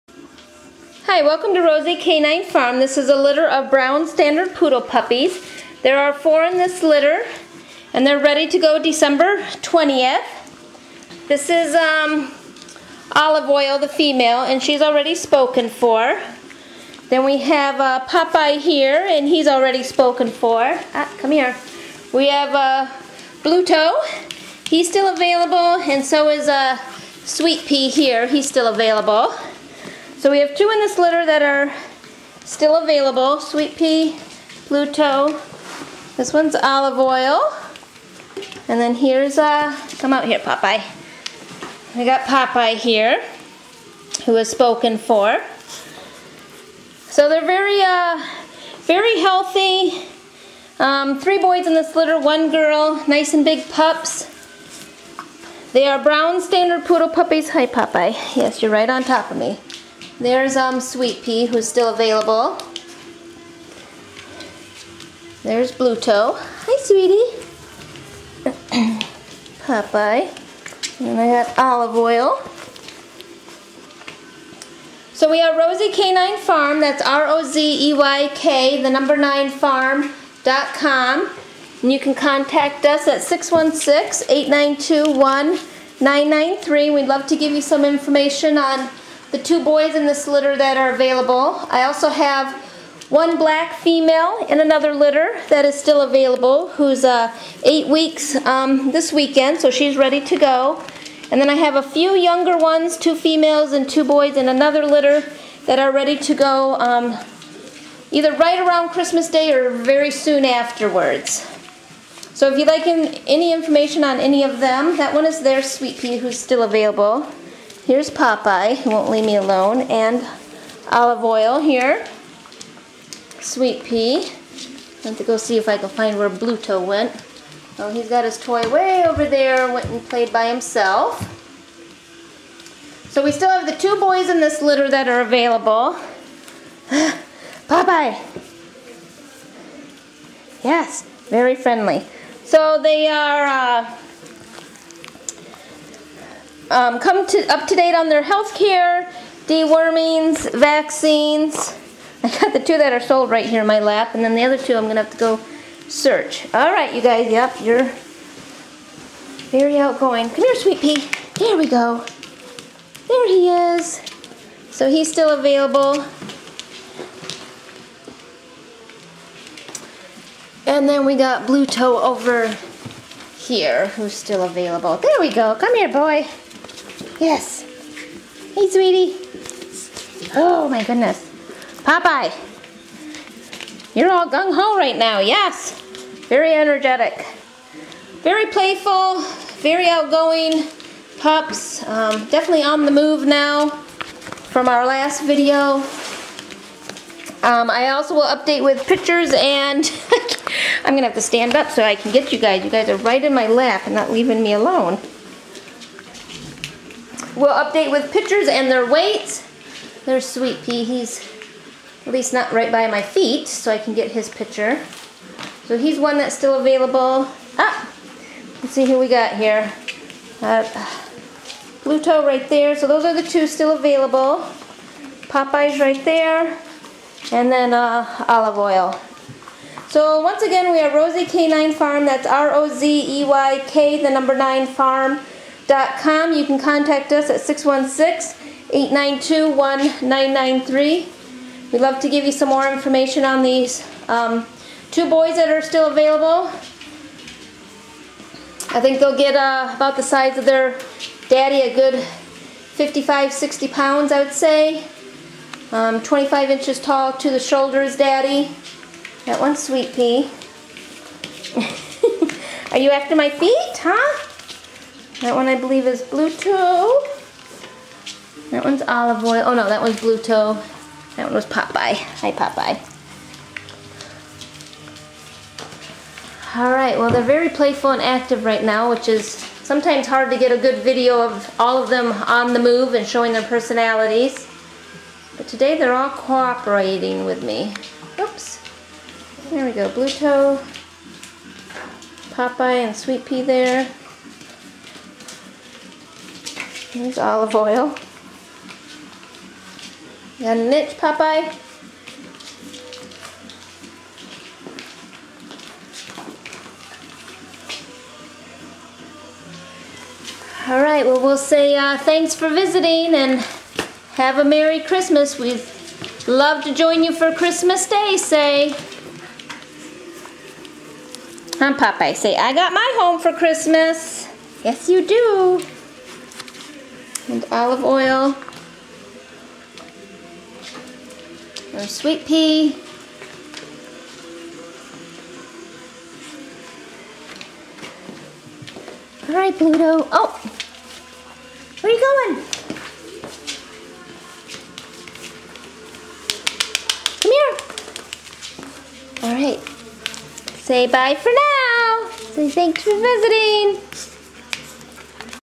AKC standard poodle puppies
Rozey K9 Farm.  Standard poodle puppies for sale in Michigan.  Brown (chocolate) in color.